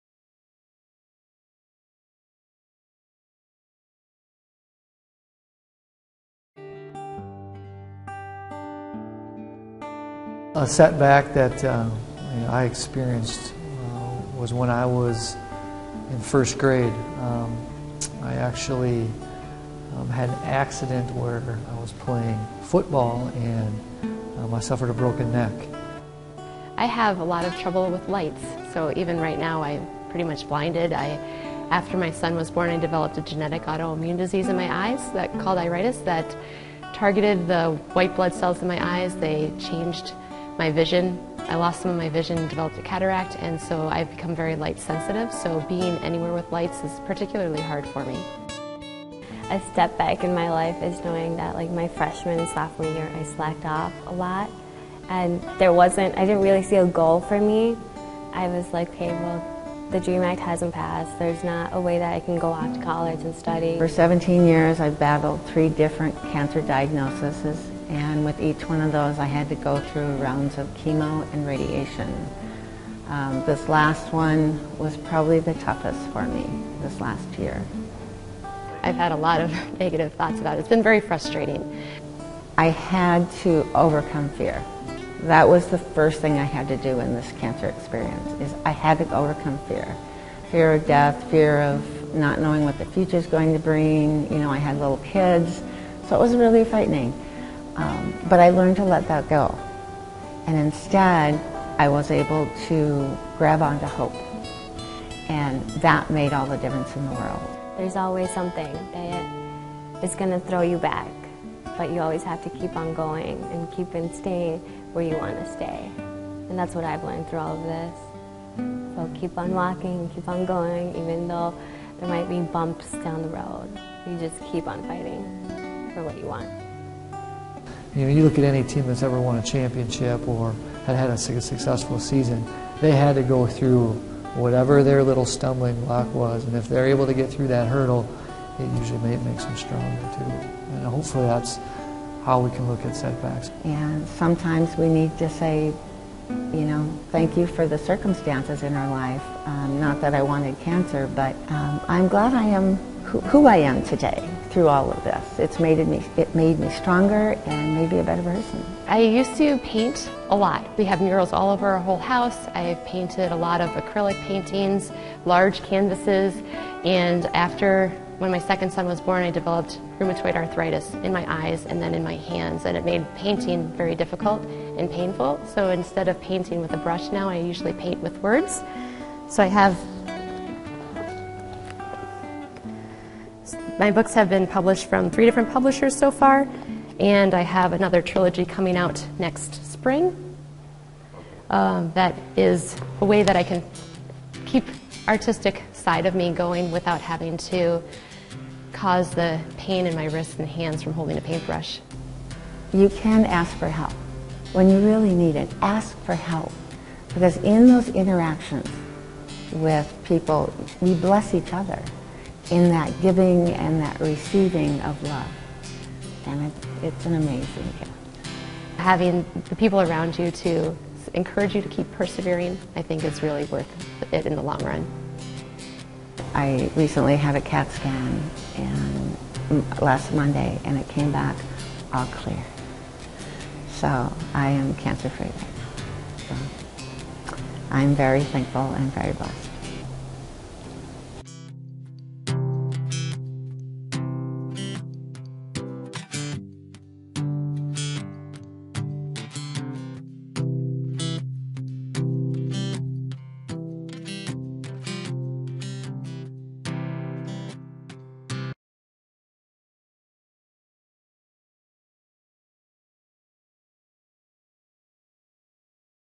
Eastview High School Extended HR: Overcoming Setbacks
Eastview High School Extended HR: Overcoming Setbacks. In a continued effort to support our culture and grow our Eastview Family - there is a continuing process of culture building and personal growth and development of our students with our faculty and staff.

Thank you to the Eastview Family for sharing of ourselves to grow our competencies to support each other to achieve and succeed in our challenging world.